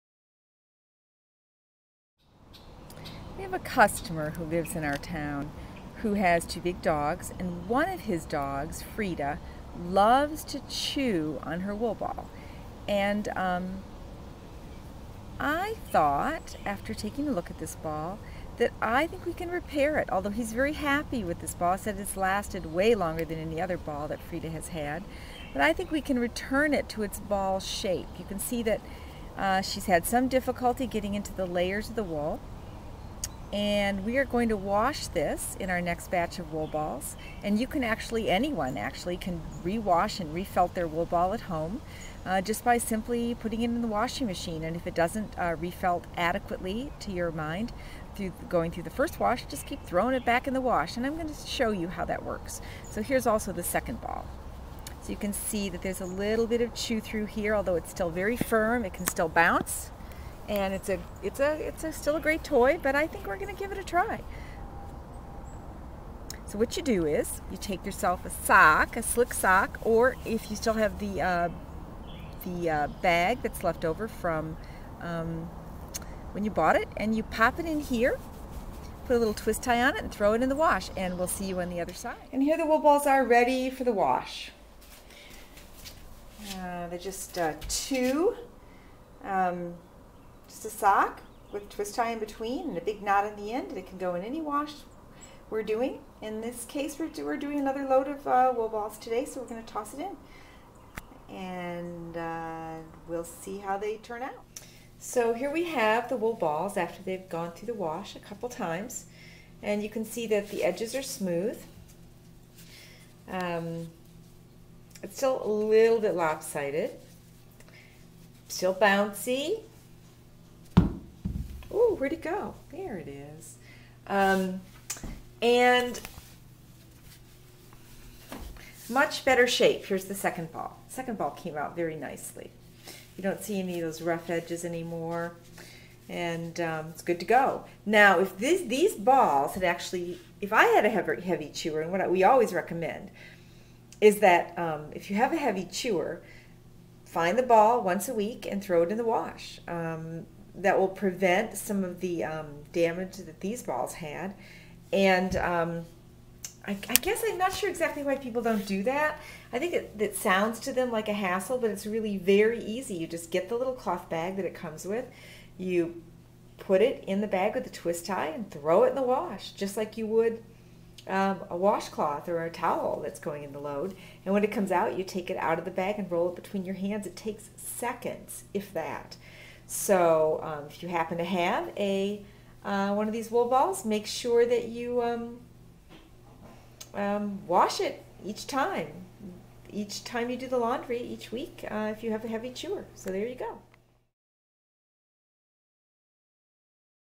New Again! Washing Purrfectplay's Organic Wool Balls
We have a customer who lives in our town who has 2 big dogs. One of his dogs Frieda loves to chew on her wool ball.  I thought after taking a look at this ball that I think we can repair it. Although he's very happy with this ball. He said it's lasted way longer than any other ball that Frieda has had.  I think we can return it to its ball shape. You can see that she's had some difficulty getting into the layers of the wool and we are going to wash in our next batch of wool balls. And you can, actually, anyone actually can rewash and re-felt their wool ball at home just by simply putting it in the washing machine and if it doesn't re-felt adequately to your mind after going through the first wash, just keep throwing it back into the wash.  So here's also the second ball. You can see that there's a little bit of chew through here although it's still very firm. It can still bounce and it's still a great toy. But I think we're going to give it a try.
So what you do is take yourself a sock or if you still have the bag that's leftover from when you bought and you op it in here, throw it in the wash and we'll see you on the other side.  And here the wool balls are, ready for the wash. There's just two. Just a sock with a twist tie in between and a big knot on the end and it can go in any wash we're doing. In this case we're doing another load of wool balls today. So we're going to toss it in and we'll see how they turn out.
So here we have the wool balls after they've gone through the wash a couple of time and you can see that the edges are smooth. It's still a bit lopsided. Still bouncy. Oh and where did it go?  There it is and much better shape. Here's the second ball, the second ball came out very nicely. You don't see any rough edges anymore and it's good to go.  Now if these balls had actually.  If I had a heavy chewer, we always recommend is that if you have a heavy chewer, we always recommend is that if you have a heavy chewer find the ball once a week and throw it in the wash. That will prevent some of the damage that these balls and I guess I'm not sure exactly why people don't do that. I think it sounds to them like it's a hassle. But it's very easy, you just get the little bag that it comes with, you put it in the bag, with the twist tie, and throw it in the wash just like you would a washcloth or a towel  that's going in the load. And when it comes out you roll it between your hands. It takes seconds if that. So if you happen to have one of these wool balls make sure that you wash it each time. Each time that you do the laundry, each week if you have a heavy chewer. So there you go.